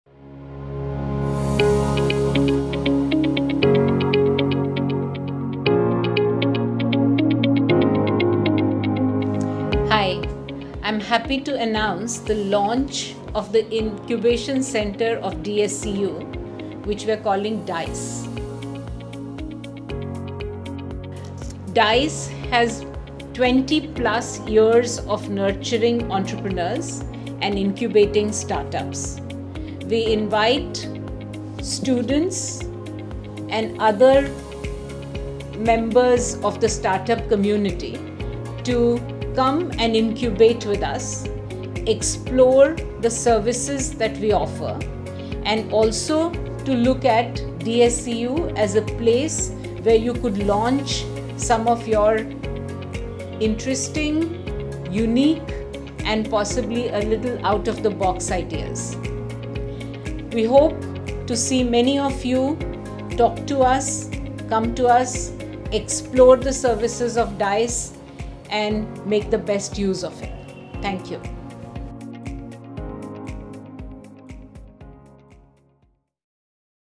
The Launch of "DSEU Innovation and Incubation Center for Entrepreneurship"
DSEU proudly launches DSEU Innovation and Incubation Centre for Entrepreneurship (DIICE). At DIICE we aim to provide a platform for students, faculty, founders of startups and businesses to develop a community of aspiring entrepreneurs. DIICE will incubate and translate interesting start-up ideas into full-fledged business. Supporting the job creators is one of our ways to help build the nation.